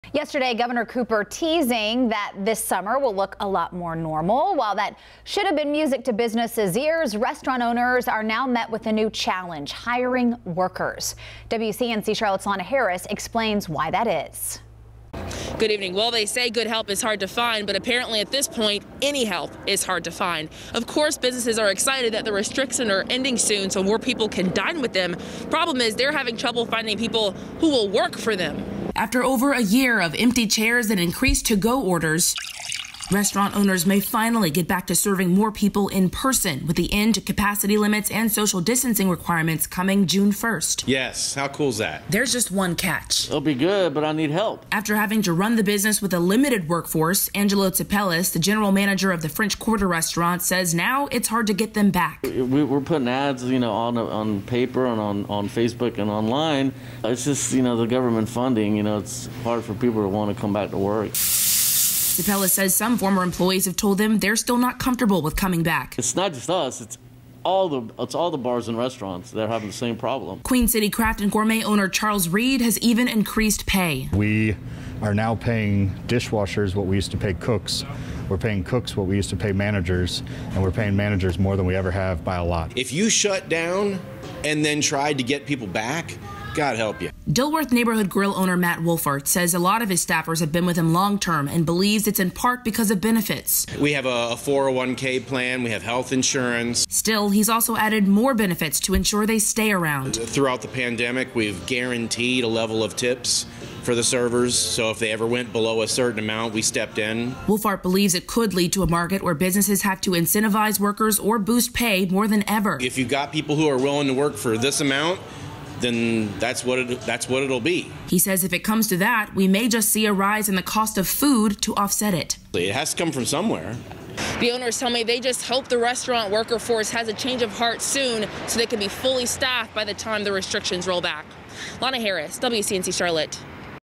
Bar and restaurant owners in Charlotte face worker shortage
What should have been welcome news of the governor revoking capacity and distancing restrictions in June has become yet another hurdle for restaurant owners.

"It’ll be good, but I need help,” said Angelo Tsepelis, the general manager of French Quarter Restaurant.

After operating with a limited staff over the pandemic, Tsepelis said now it's hard to get them back.

"We’re putting ads on paper and on Facebook and online," Tsepelis said. "You know with government funding, it's hard for people to want to come back to work."

Tsepelis said some former employees have told them they're still not comfortable with coming back.

"It’s not just us, it's all the bars and restaurants," Tsepelis said, "they're having the same problem.”